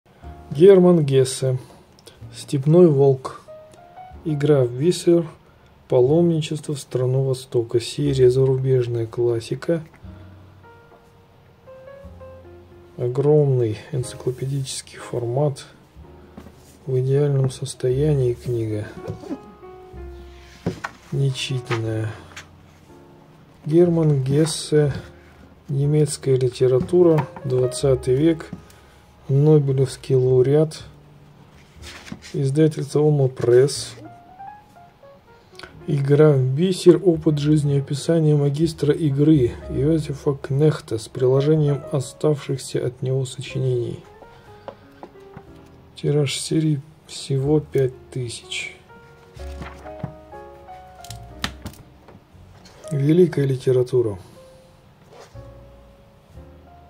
Герман Гессе. Степной волк; Игра в бисер
№3444. Гессе Герман. Степной волк; Игра в бисер
Пер. с нем. С. Апта
Серия: XX век. Зарубежная классика
Москва. Олма-Пресс. 2004г. 605c
Твердый переплет. Энциклопедический формат
Состояние:  Отличное (экз. не читан)

В книге представлены два романа и повесть выдающегося немецкого писателя, нобелевского лауреата по литературе 1946 года. Его творчество открыло новые перспективы перед интеллектуальным гуманизмом.

Смотрите книгу в нашем каталоге в Telegram-канале OldbookKz.
Букинистические книги в Казахстане. Высылаем почтой.